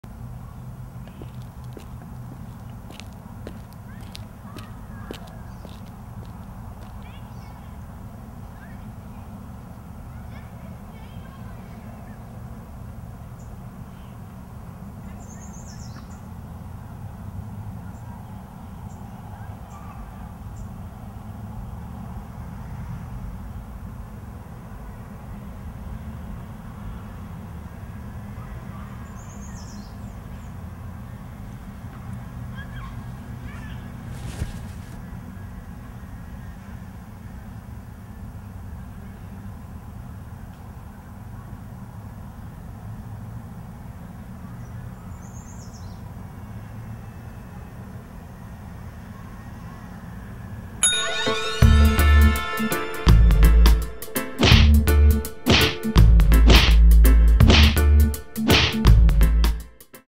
Sweet Stix Commercial
A commercial our company put together for Guitars Unplugged at BYU-Idaho.